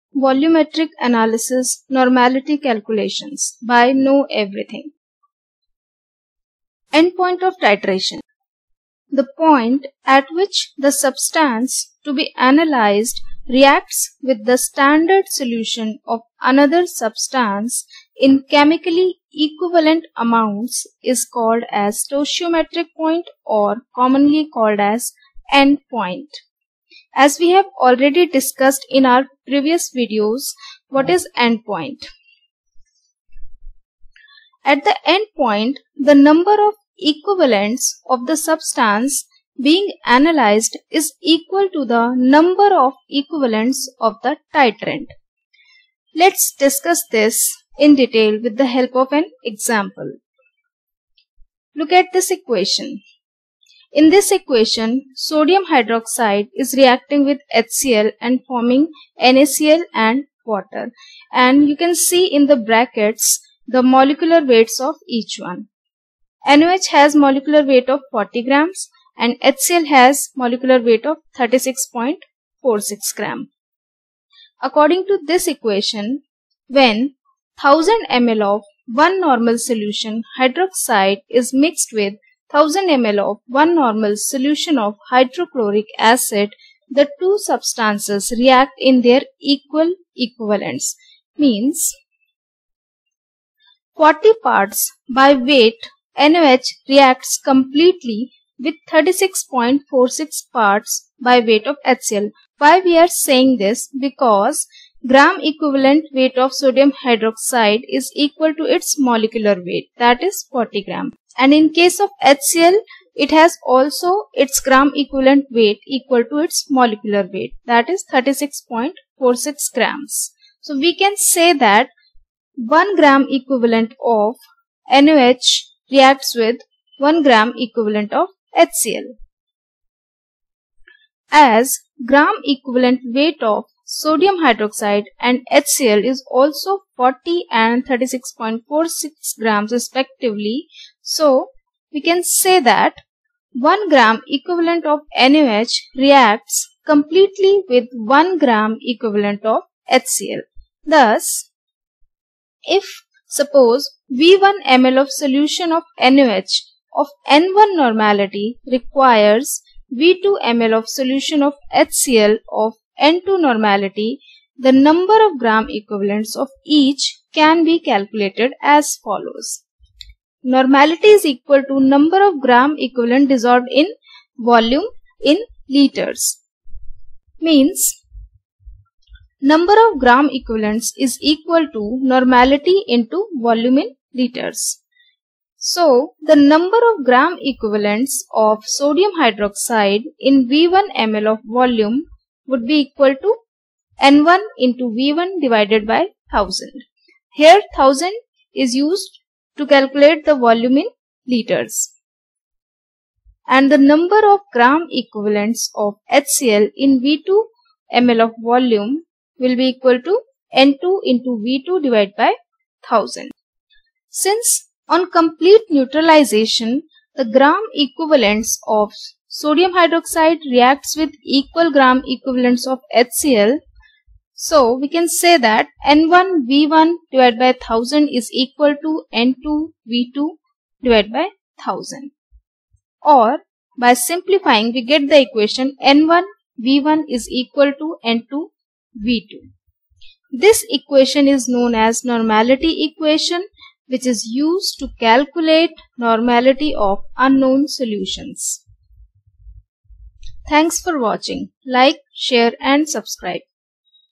Analytical Chemistry: Volumetric Analysis : Normality Calculations explained with example
In Analytical chemistry, Volumetric analysis is the determination of a solution of known concentration which is required to react quantitatively with solution of the substance being analysed.
The strength of solution is expressed as normality and molarity.
this video is about how normality equation is formed and used.